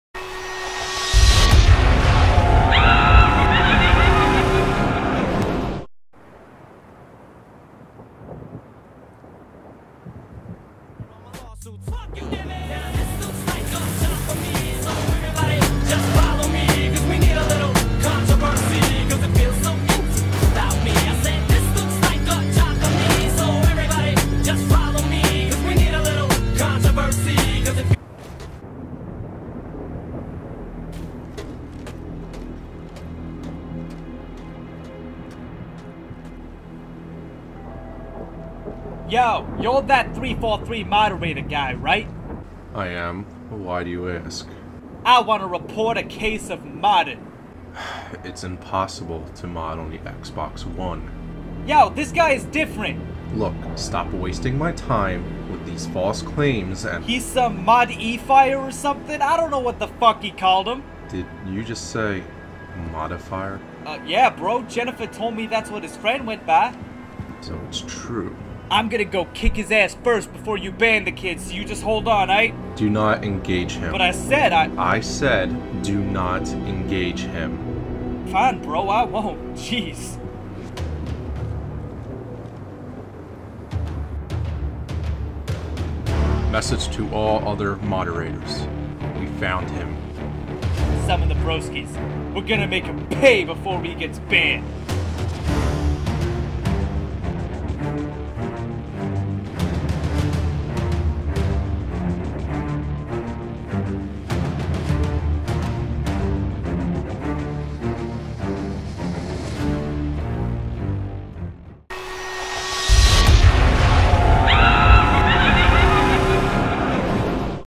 The Professional: Modefier Saga (EP. 2.5)
Oh snap. That sStruddle wannabe tryhard just ratted out Mod E. Fier to the 343 police. Things are about to get edgy, bro.

Gamertag: HC Tylar

Gamertag: HC BlameRickey